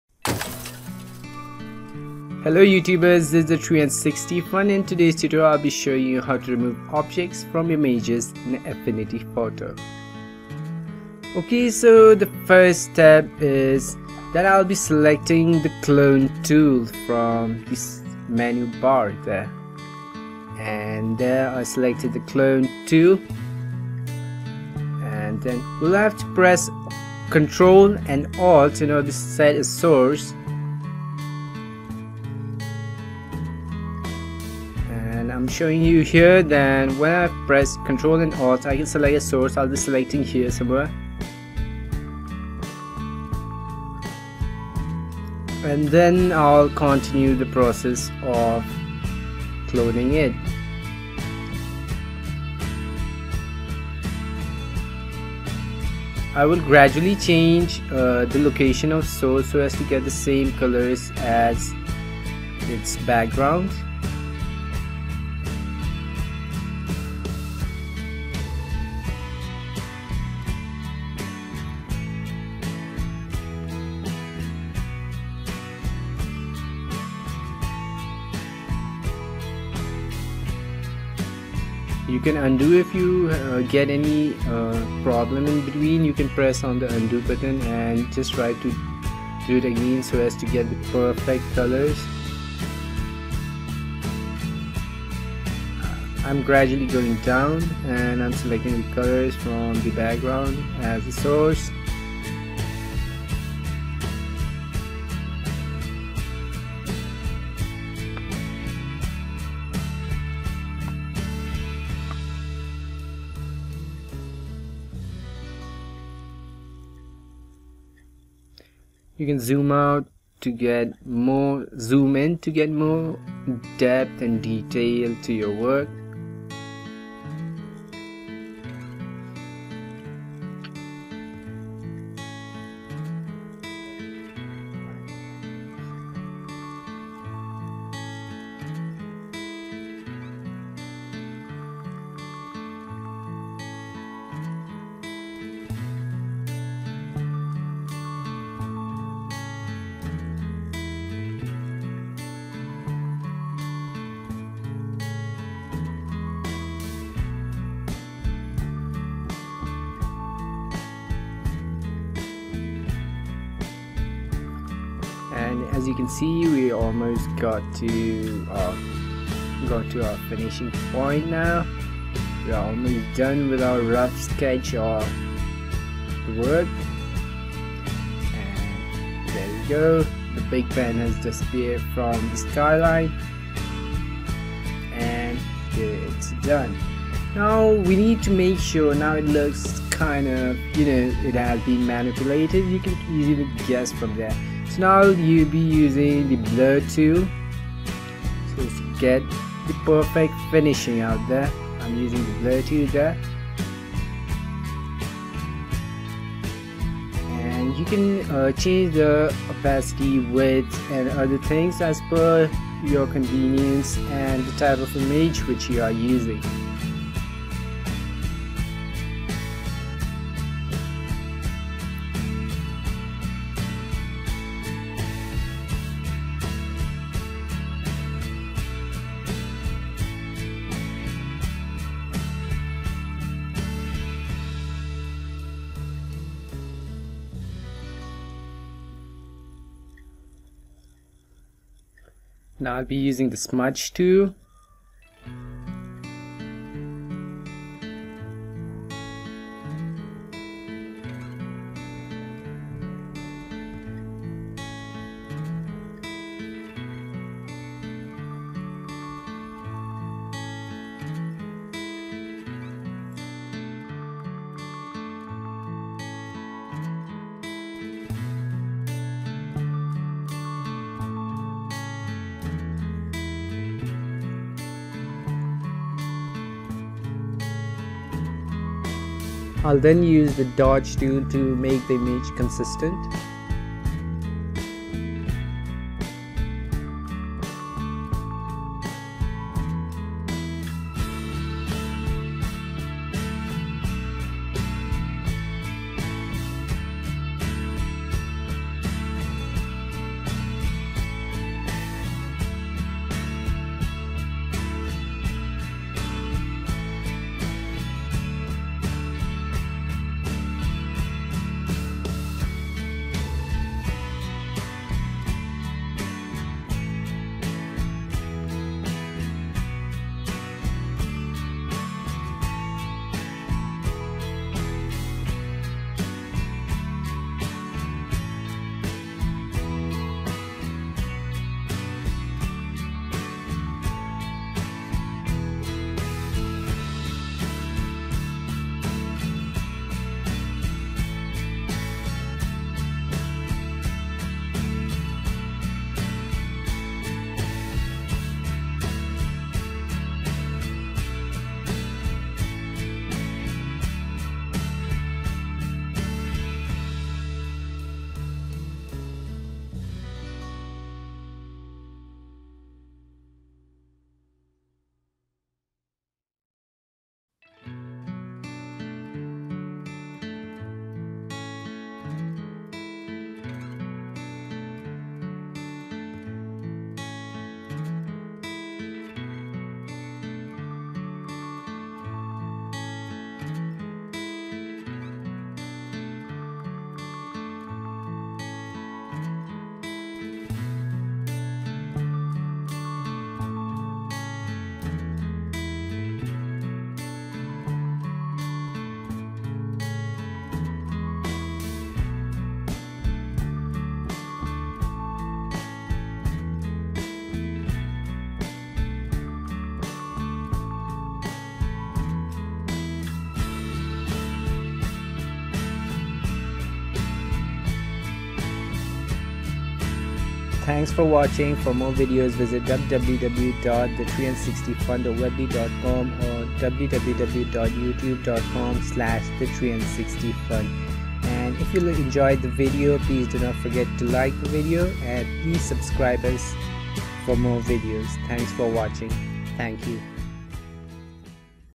Removing objects from images in Affinity Photo
This video shows you how to remove objects from images in Affinity Photo.
Affinity Photo is a proprietary photo editor for Apple macOS and Microsoft Windows by Serif Europe.